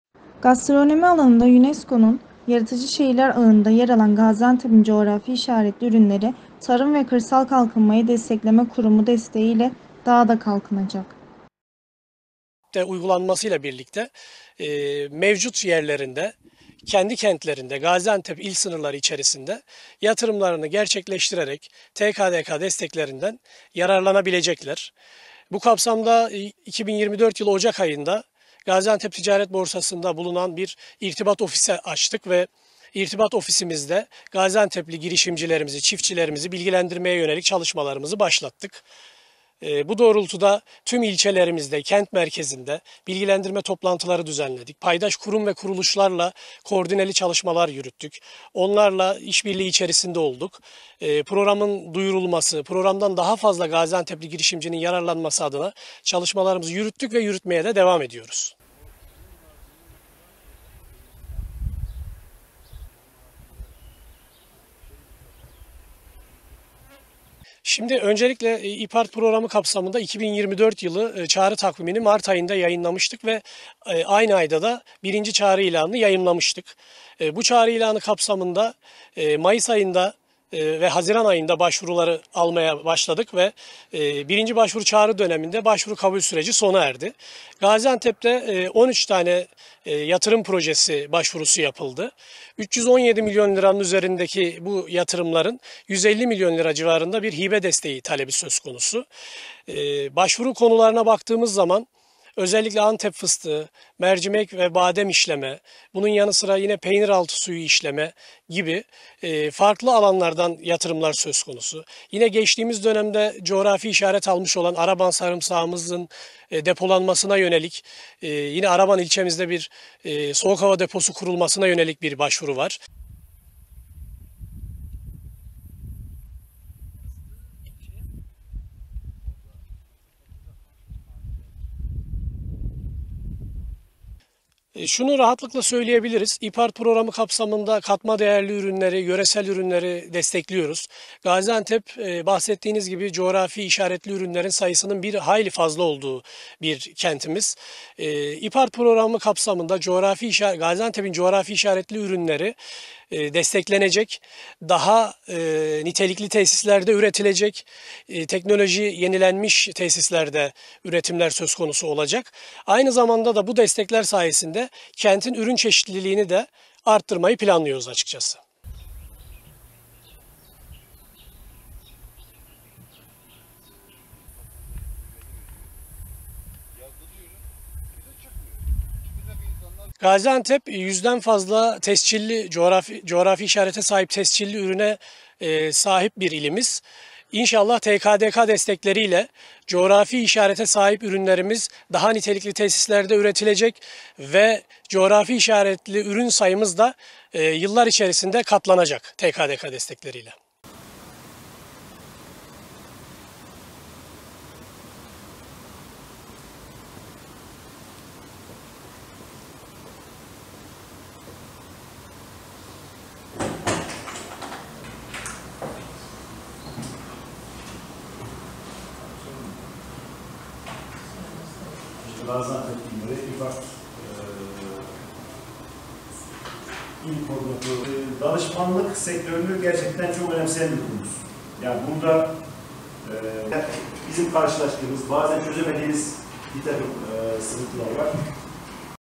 Coğrafi İşaretli Ürünleri TKDK İle Kalkınacak
Kentin bu yıl TKDK desteklerine dahil edilmesiyle IPARD III Programı'nın ilk çağrısına 13 fiziki proje başvurusu yapıldı